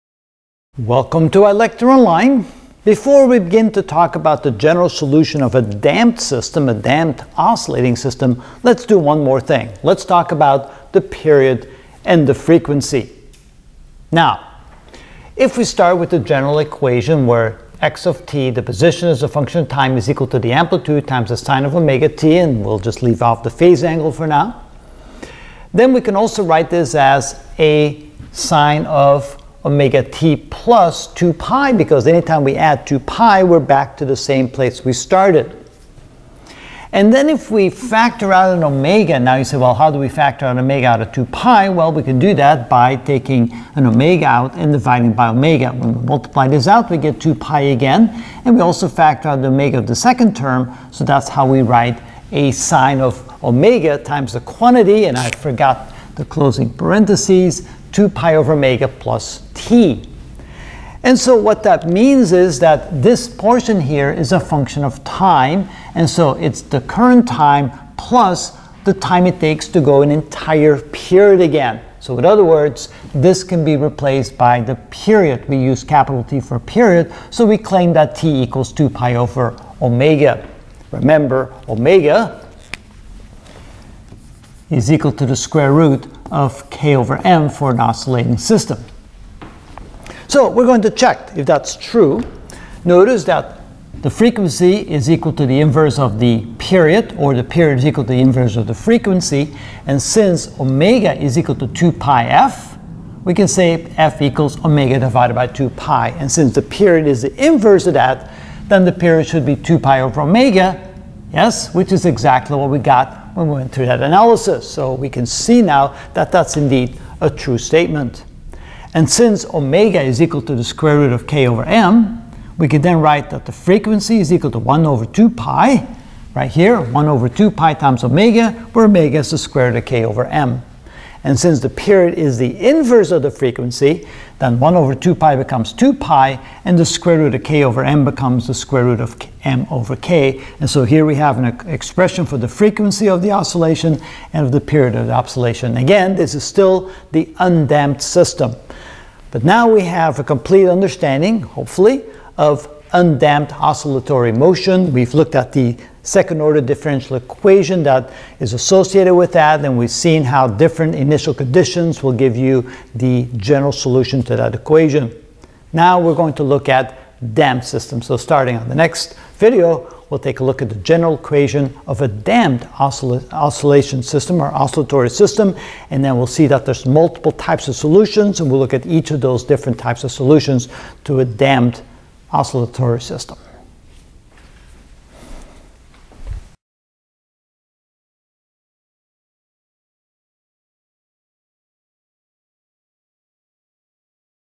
Physics: Ch 16.1 Simple Harmonic Motion with Damping (6 of TBD) Period and Frequency
We are going to talk about the period and frequency from the general equation x(t)=Asin[(omega)t] of an undamped simple harmonic motion.